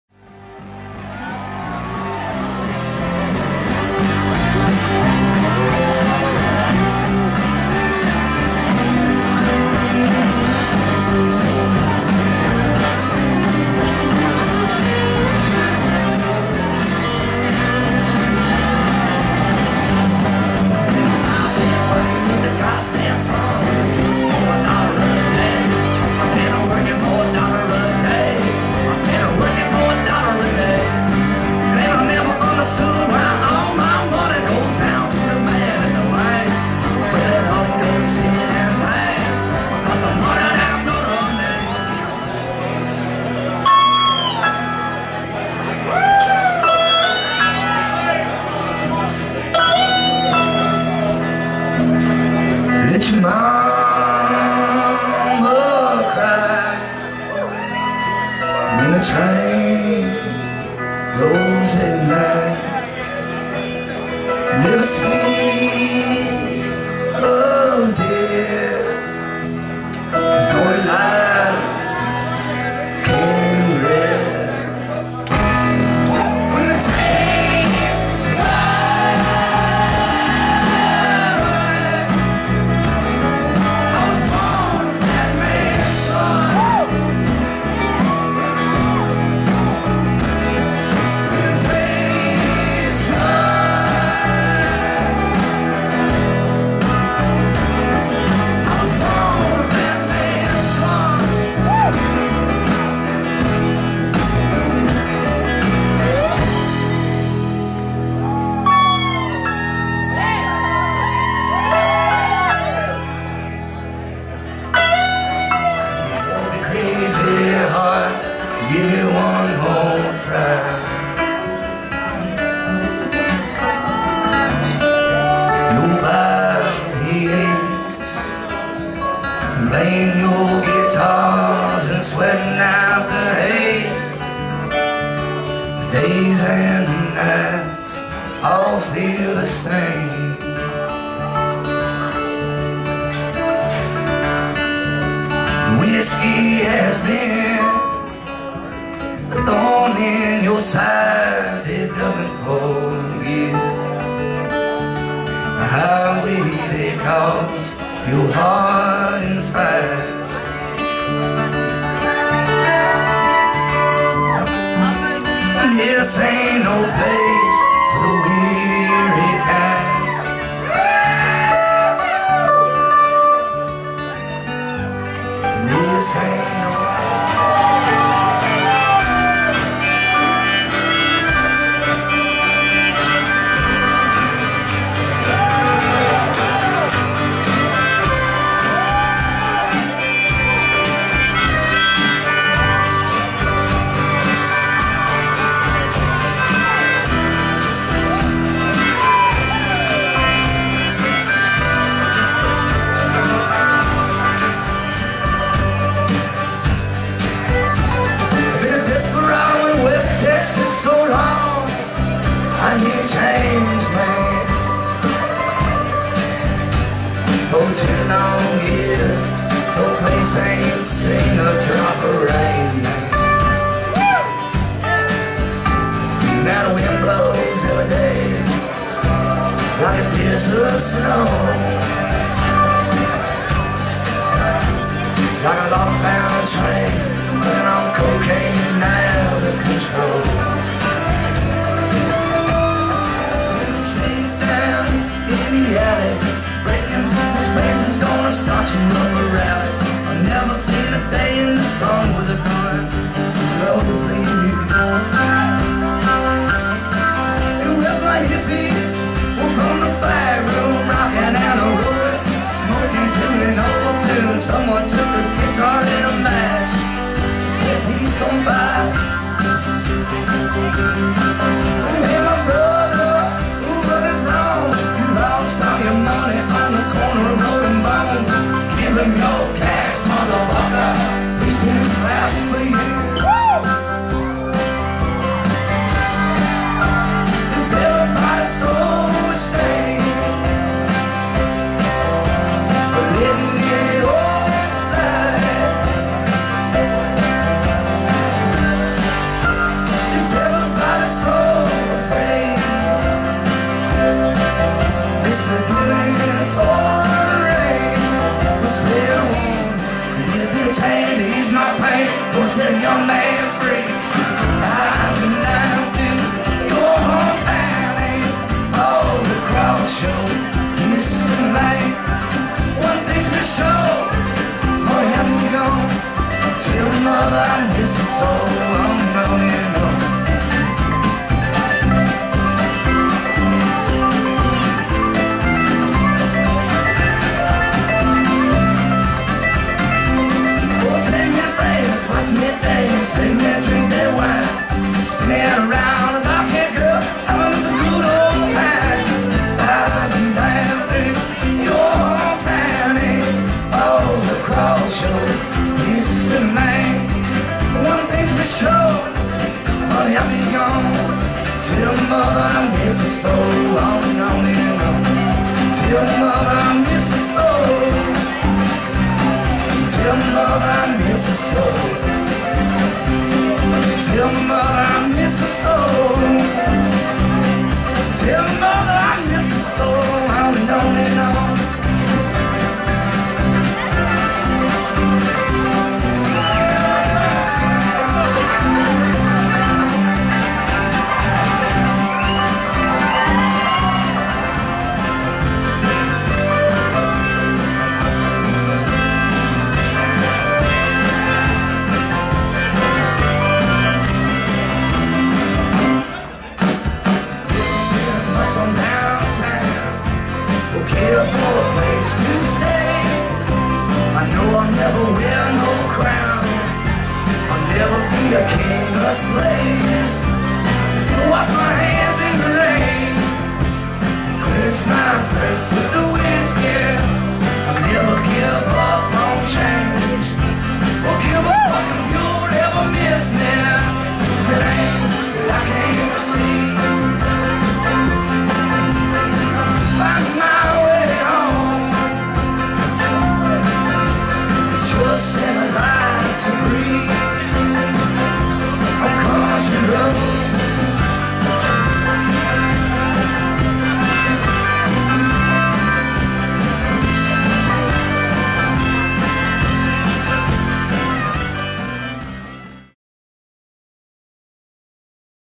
Ryan Bingham - May 2010 - Smith's Olde Bar - Dead Horses
Ryan and his band The Dead Horses in Atlanta.  Montage of nearly 2 hours of music at Smith's Olde Bar.